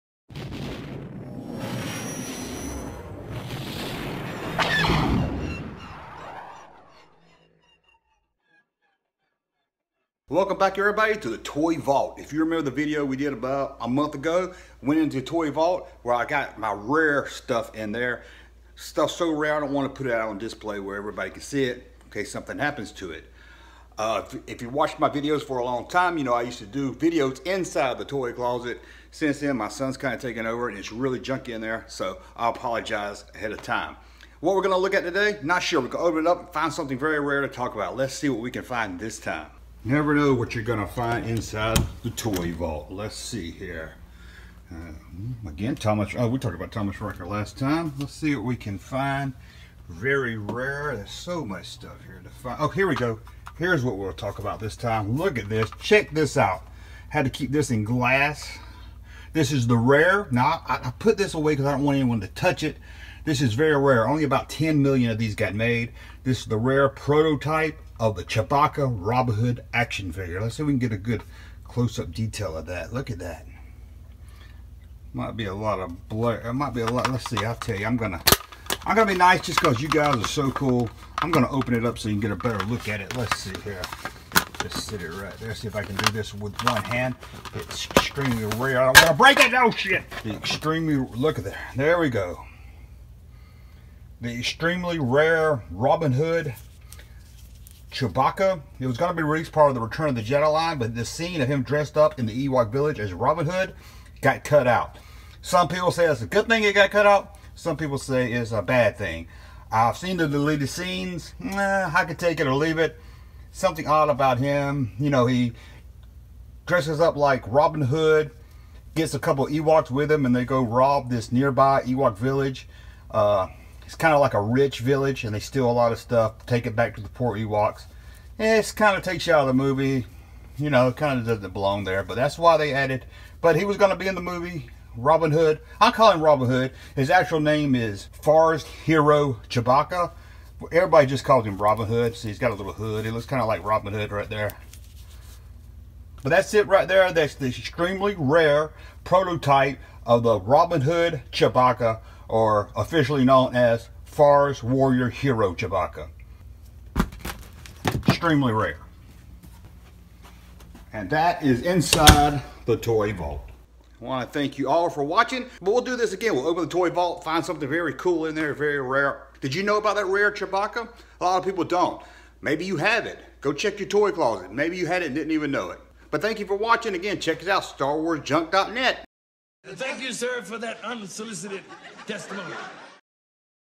Inside the Toy Vault: Episode 2
We return of a look inside the Toy Vault! This time the rare "Robin Hood Chewbacca!